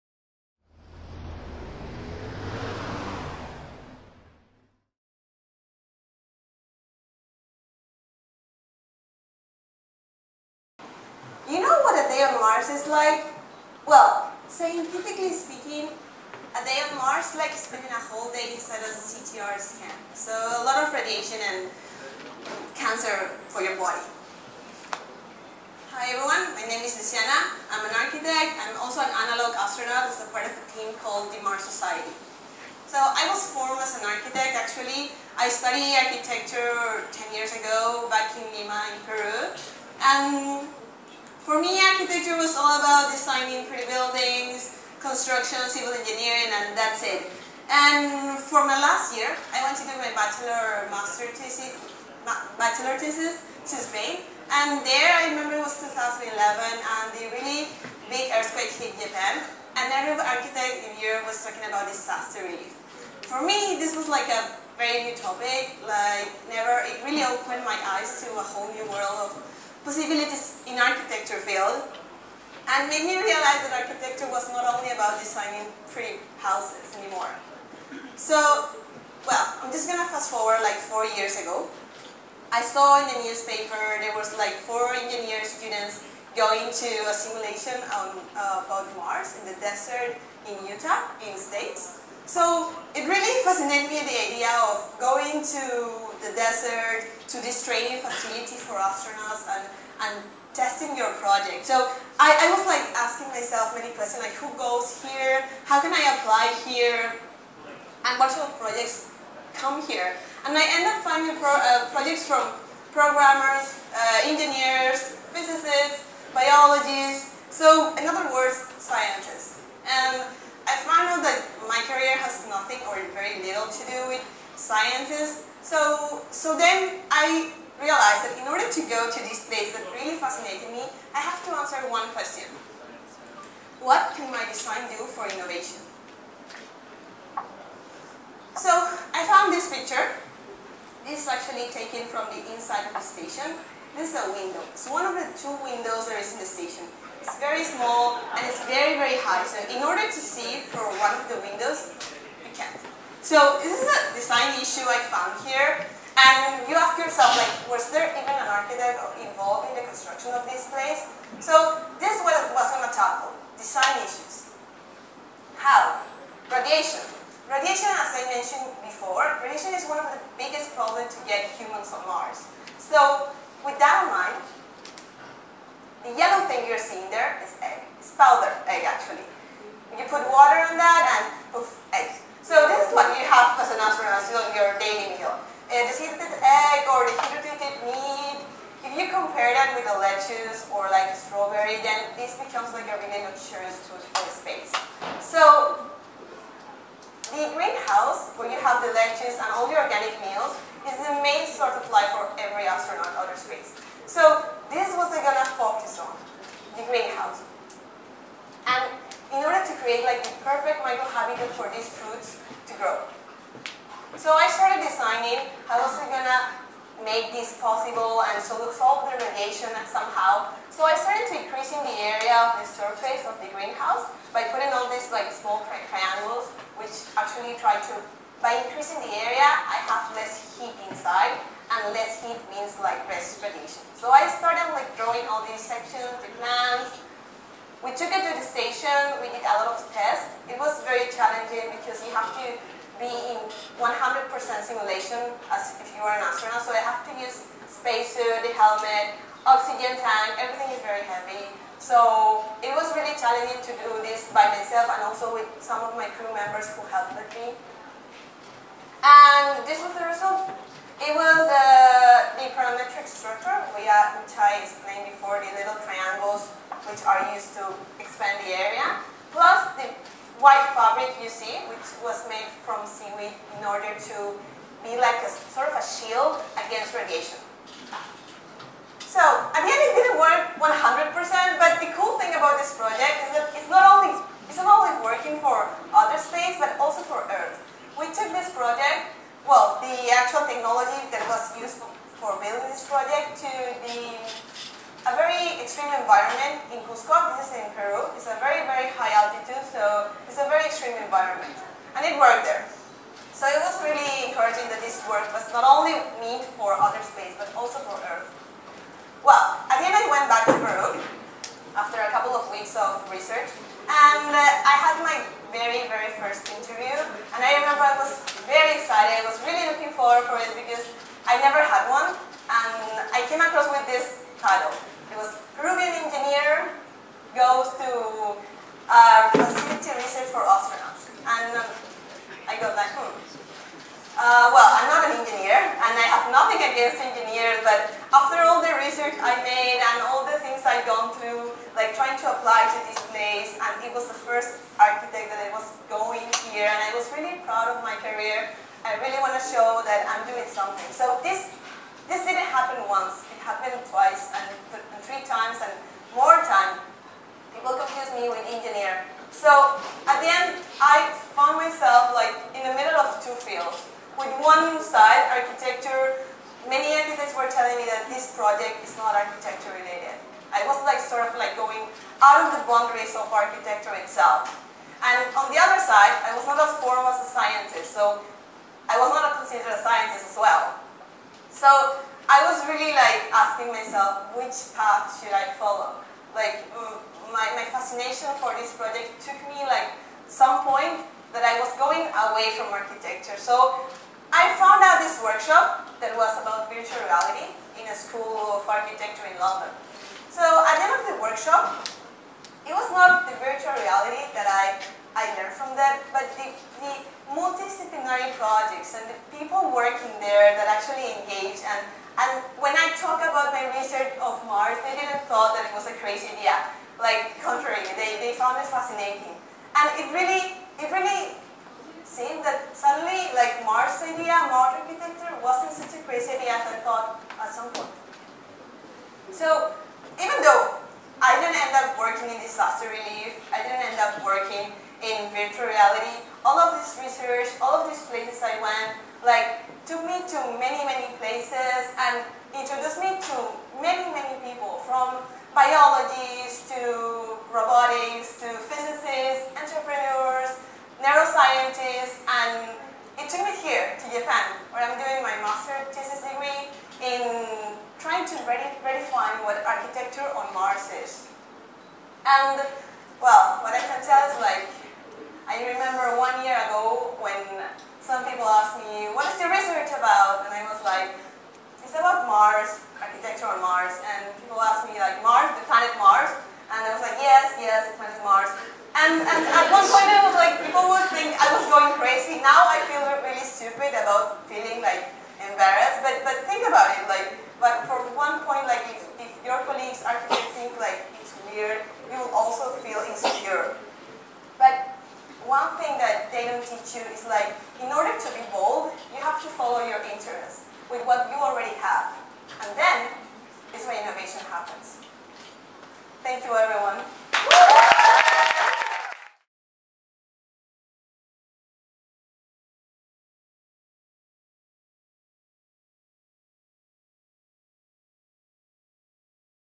Creating a Space for Life on Mars | Luciana Tenorio | TEDxYouth@Tokyo
Can you imagine how you'd live your life on Mars? Luciana is an acclaimed architect currently doing research on technology application in architecture, to develop better living environments for people in the future. In her talk, she will guide us through her ideas of building a UV insulating greenhouse to grow fresh food in an environment like Mars that she tested out at the Mars Desert Research Station in Utah. Luciana is an acclaimed architect currently doing research on technology application in architecture, to develop better living environments for people in the future. This talk was given at a TEDx event using the TED conference format but independently organized by a local community.